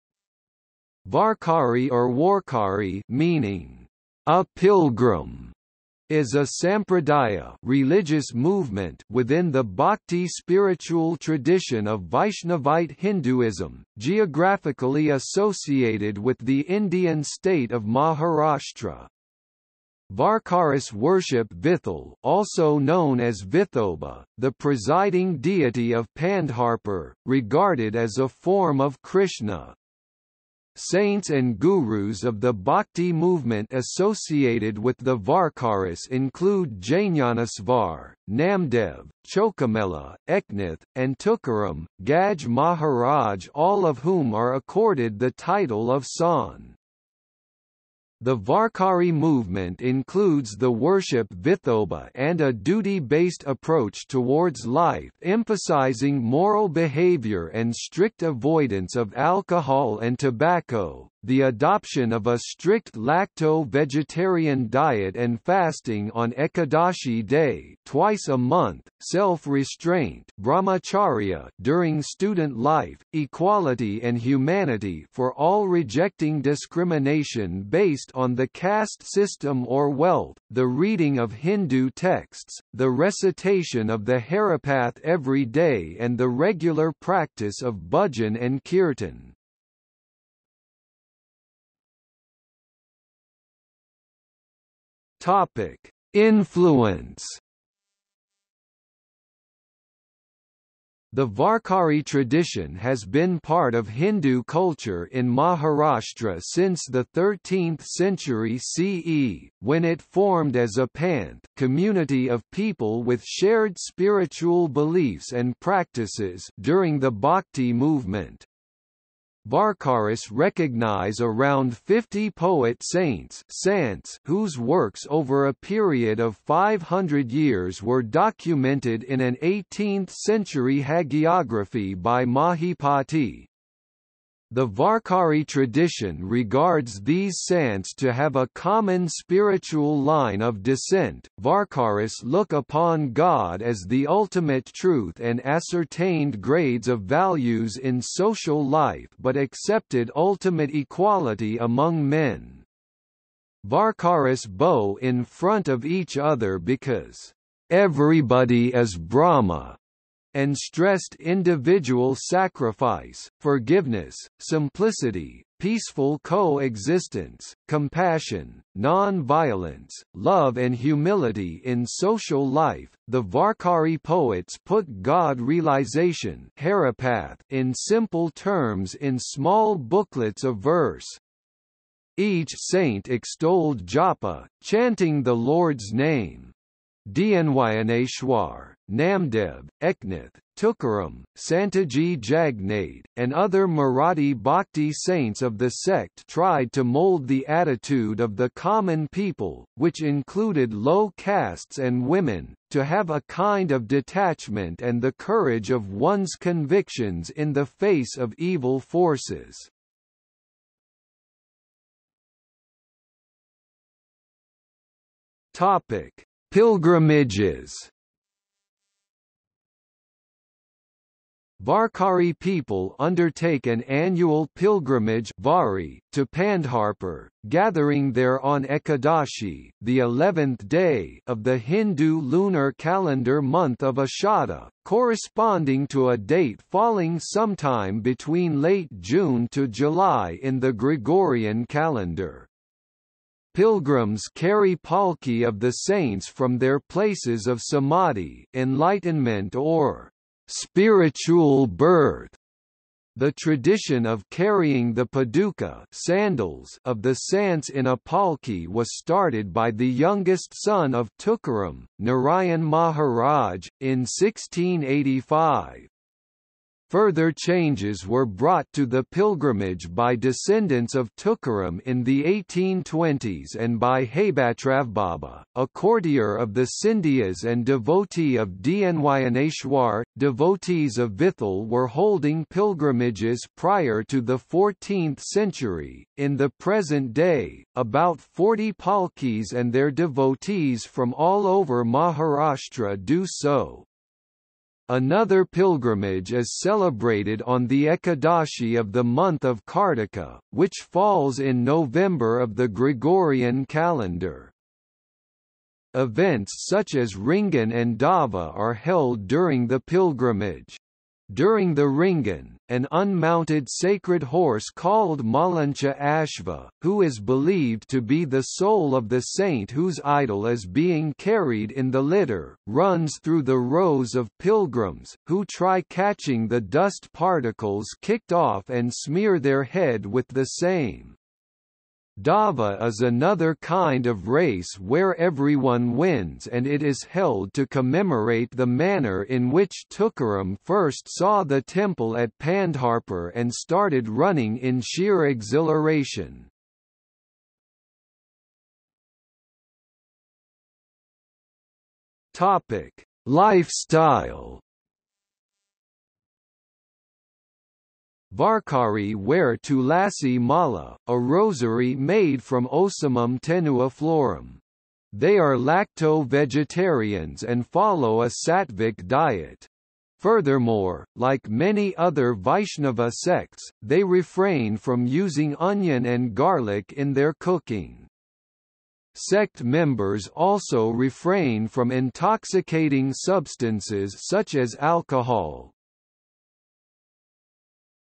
Varkari | Wikipedia audio article
00:01:31 1 Influence
00:03:31 2 Pilgrimages
00:05:48 3 Lifestyle

- Socrates

SUMMARY
Varkari or Warkari (meaning "a pilgrim") is a sampradaya (religious movement) within the bhakti spiritual tradition of Vaishnavite Hinduism, geographically associated with the Indian state of Maharashtra. Varkaris worship Vitthal (also known as Vithoba), the presiding deity of Pandharpur, regarded as a form of Krishna. Saints and gurus of the bhakti movement associated with the Varkaris include Jñāneśvar, Namdev, Chokhamela, Eknath, and Tukaram, Gadge Maharaj all of whom are accorded the title of Sant.
The Varkari movement includes the worship Vithoba and a duty-based approach towards life emphasising moral behavior and strict avoidance of alcohol and tobacco, the adoption of a strict lacto-vegetarian diet and fasting on Ekadashi day (twice a month), self-restraint (brahmacharya) during student life, equality and humanity for all rejecting discrimination based on the caste system or wealth, the reading of Hindu texts, the recitation of the Haripath every day and the regular practice of bhajan and kirtan.